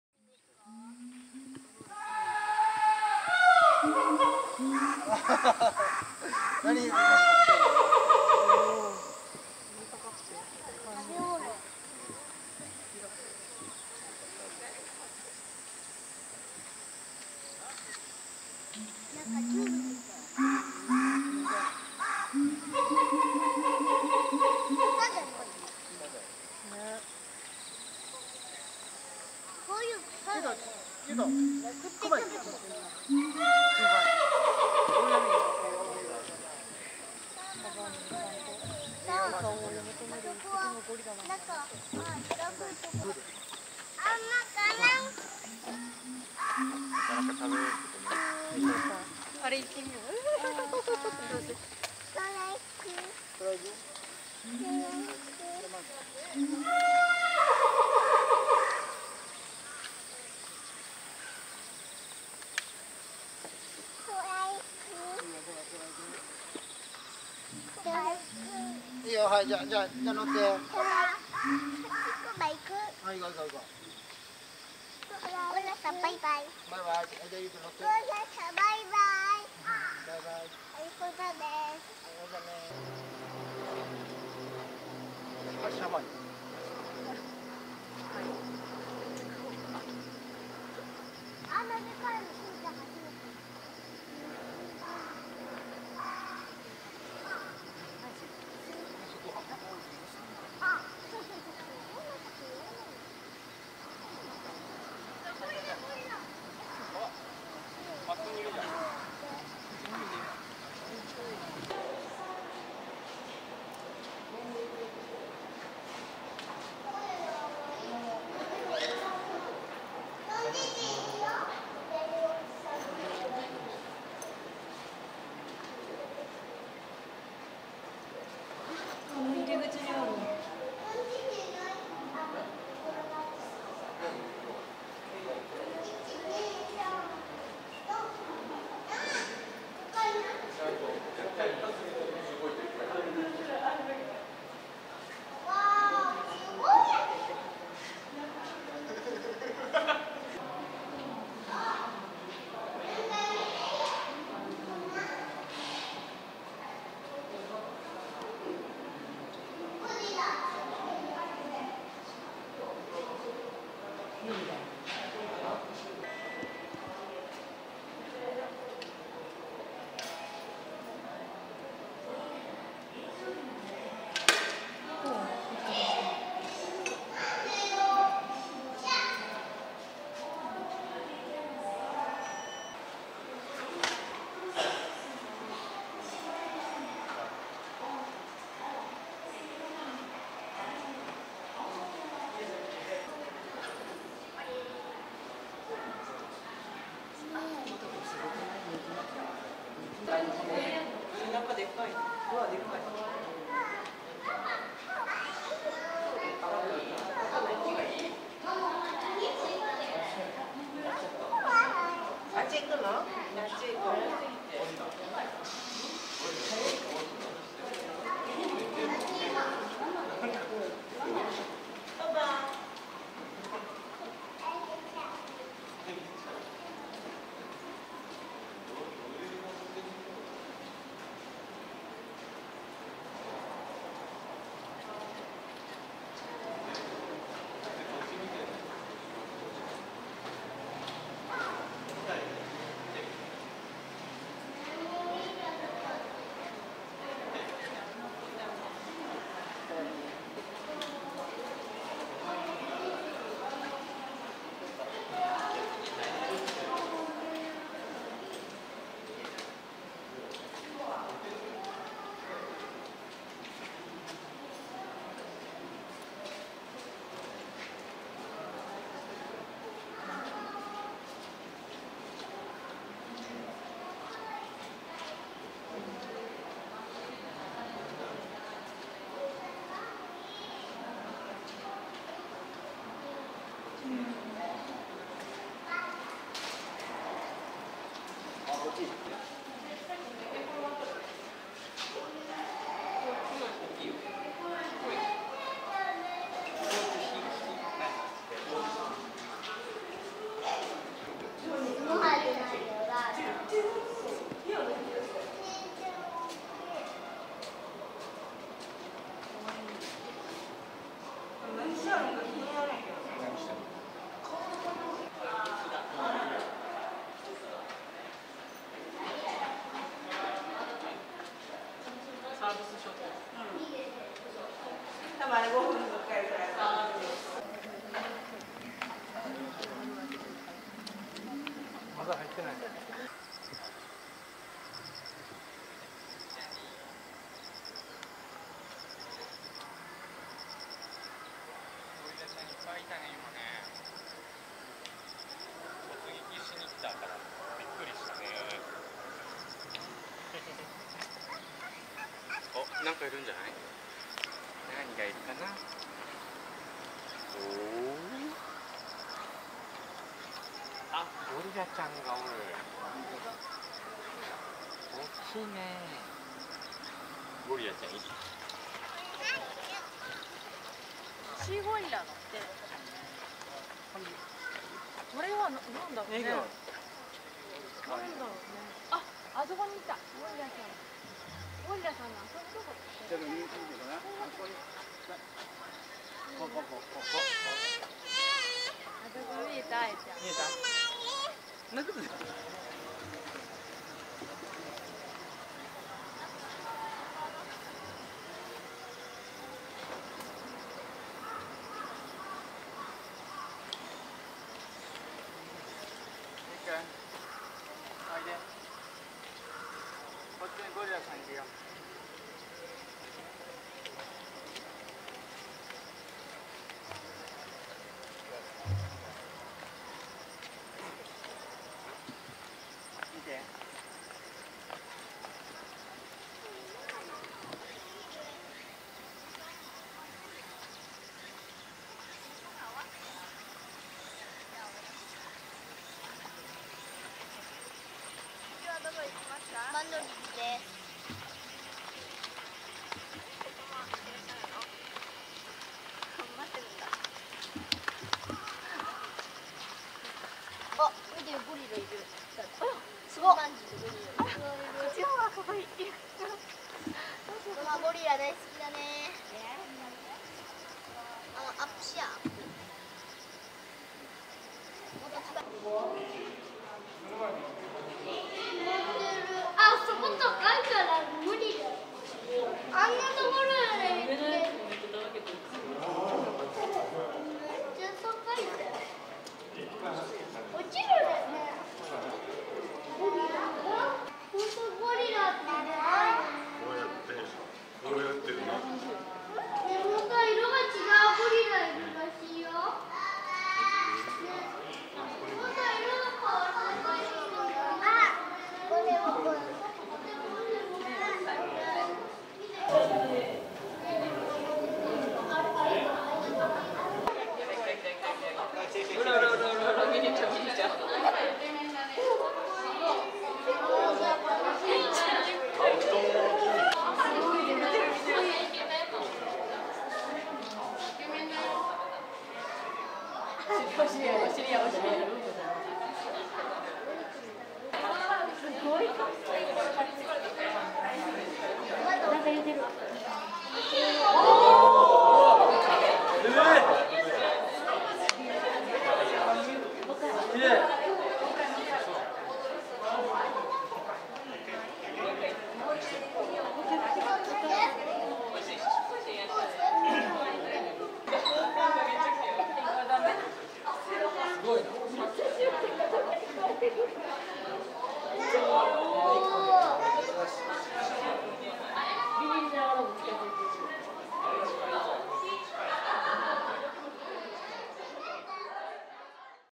鼻を動かすネネと笑顔の日曜日 〜 2023/08/27【東山動物園】
2023/08/27 日曜日

東山動植物園で撮影したニシローランドゴリラのシャバーニ一家の様子です。

2023/08/27 Sunday

This is the state of the Shabani family of western lowland gorillas taken at the Higashiyama Zoo and Botanical Gardens.